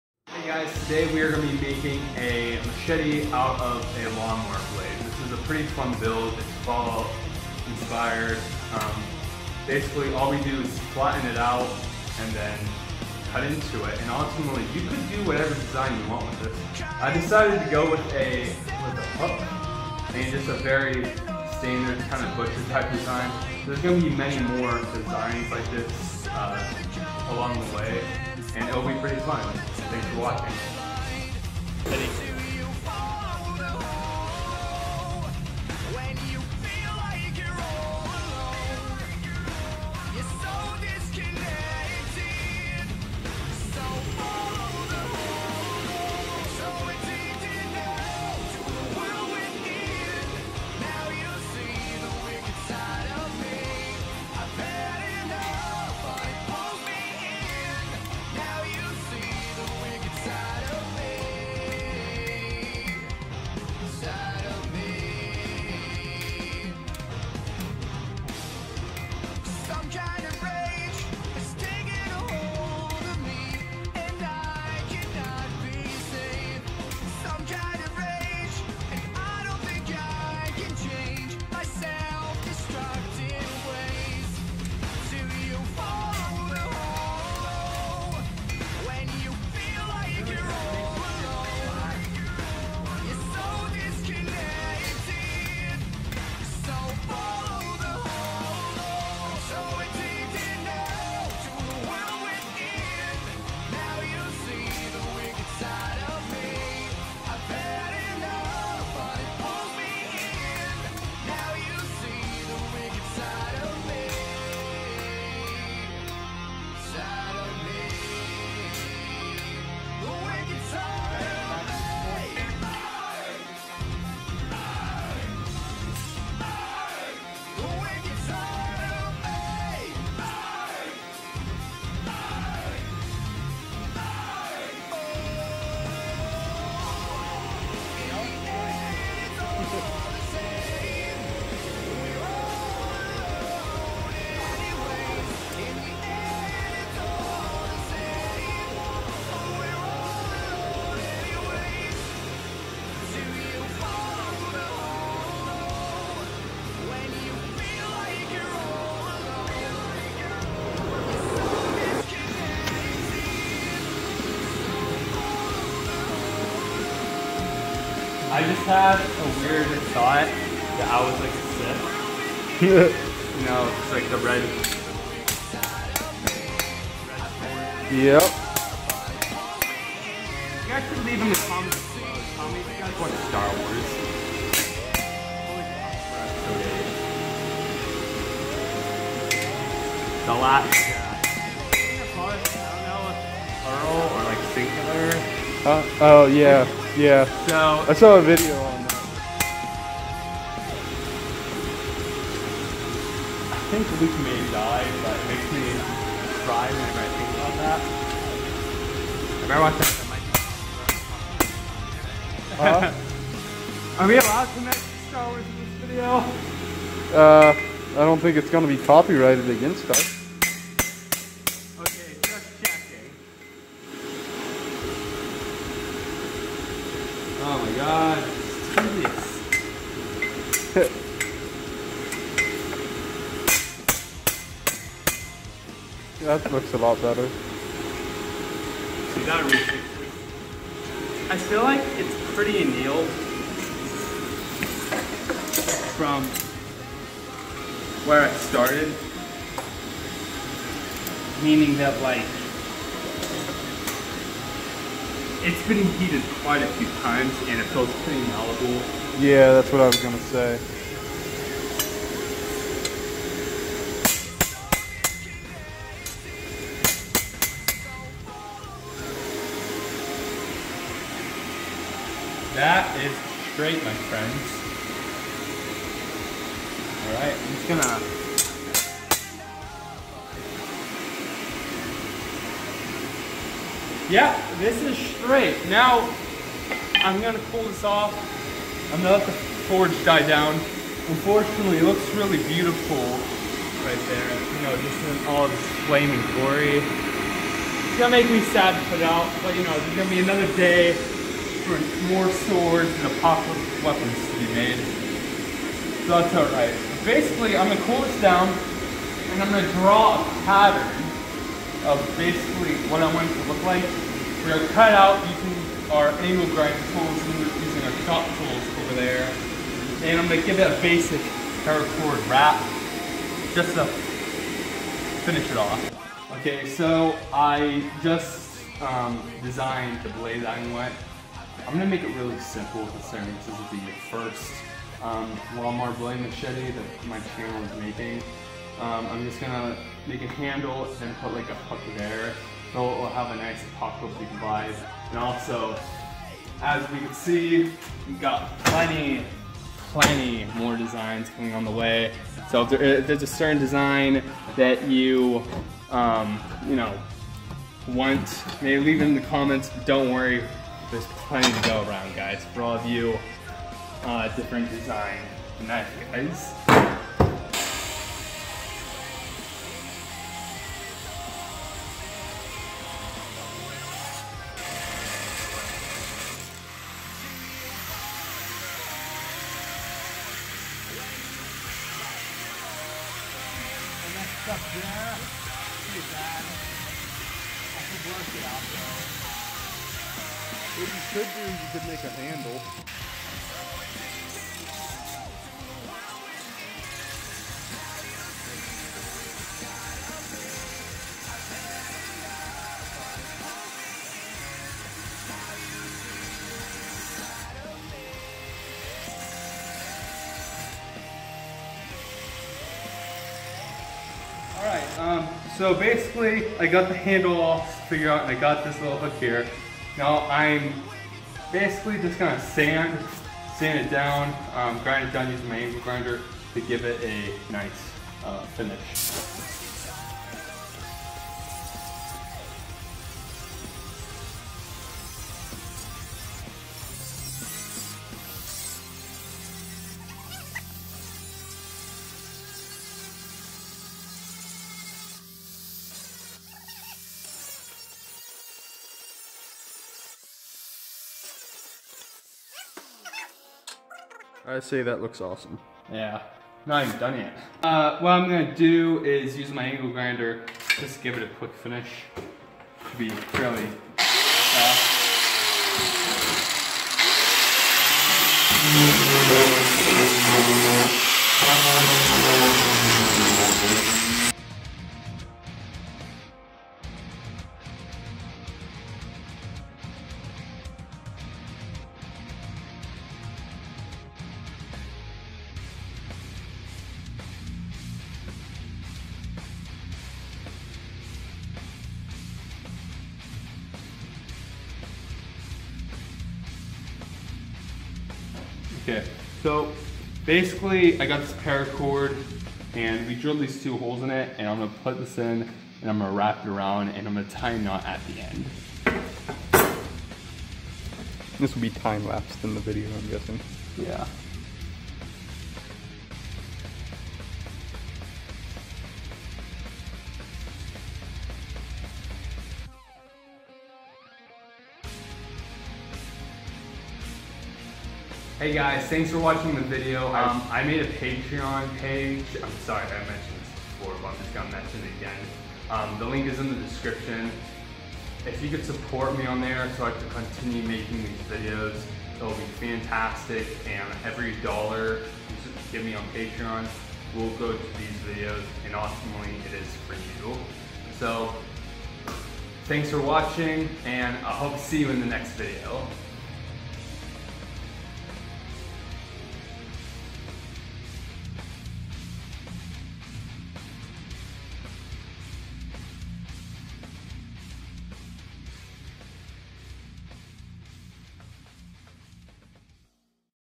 How to make an Apocalyptic Machete from a Lawnmower Blade
How to make an Apocalyptic Machete from a Lawnmower Blade.   I will be making many more videos like this but with different machete designs.